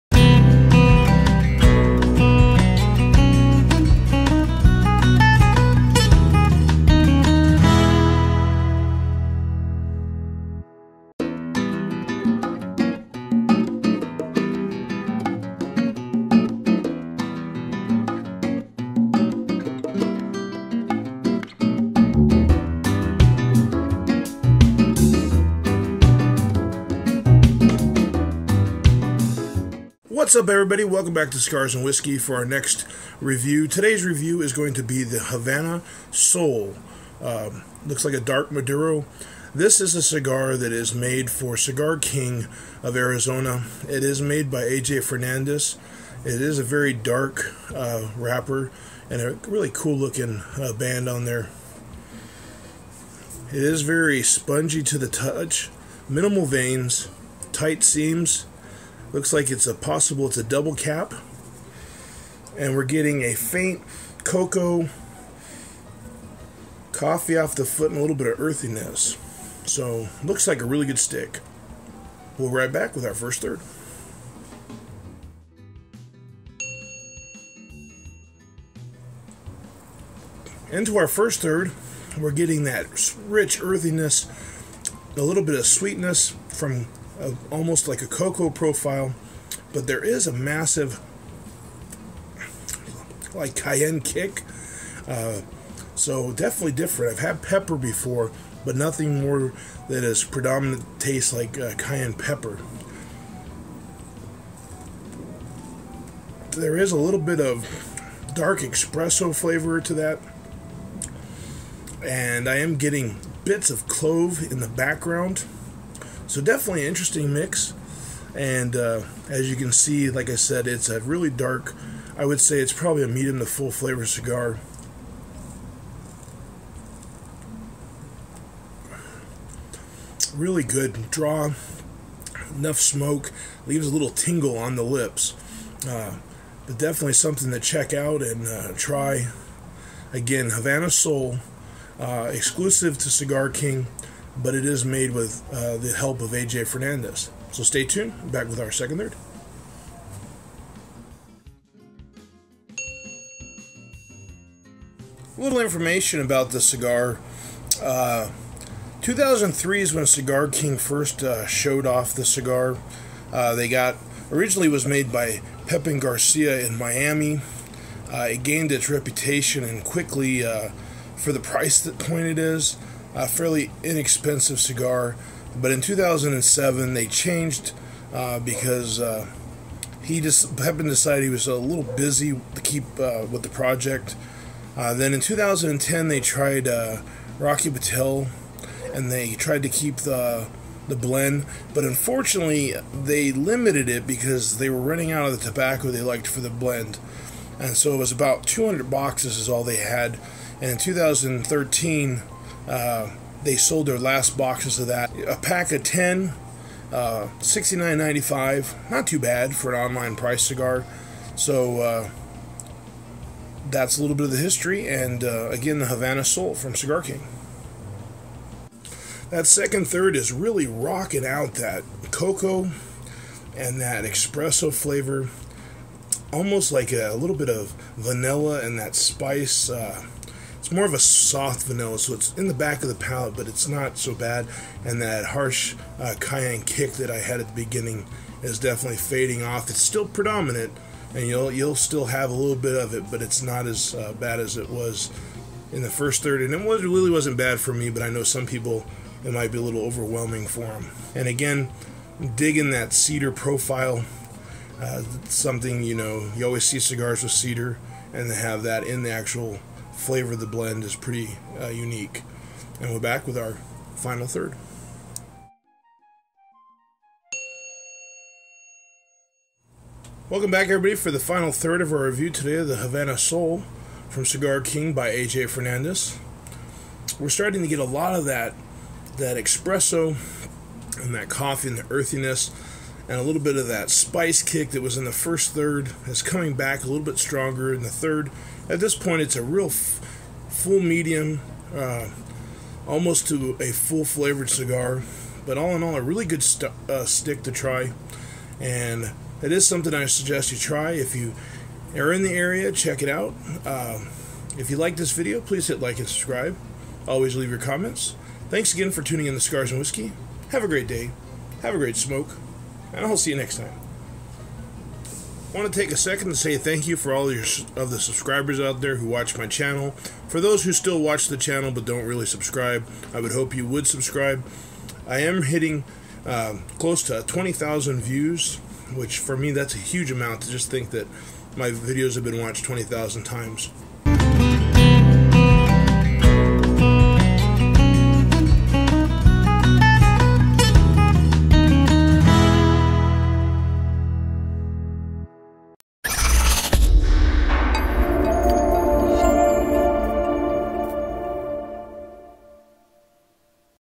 Havana Soul Cigar Review S02;Ep52
Description  By Cigar King

The Havana Soul brand has been around in a few different variations since 2003.  Originally made by Pepin Garcia in Miami, FL, Havana Soul quickly gained a reputation for being an outstanding cigar at an exceptional price.  In 2007, Pepin decided he was too busy to continue with this project and we took the brand out of production until we found a blend that was a worthy successor.  In 2010, Rocky Patel had a hot-hand and created a blend that gained even more attention than the original release.  However, he only had enough of the right tobaccos to produce a scant 200 boxes.  In 2013, we sold our last box from the Rocky Batch, but we hunted down the tobaccos used by Rocky and his former blender (and now independent cigar maker) AJ Fernandez had what we were looking for.

The NEW Havana Soul by AJ Fernandez has been blended to perfection using the same, well-aged tobaccos as before.  The format is a 6x52 Toro and will be produced in the same limited run of 200 boxes of 20. They are produced at his Tabacalera Fernandez factory in Esteli, Nicaragua using an Ecuador Habano wrapper combined with a Nicaragua binder and Nicaragua filler leaf. Once rolling is complete, the cigars are aged 12 months to ensure excellent combustion and flavor.  The NEW Havana Soul by AJ Fernandez is full bodied in strength and rich with flavors of earth, leather, and coffee with hints of cocoa on the finish.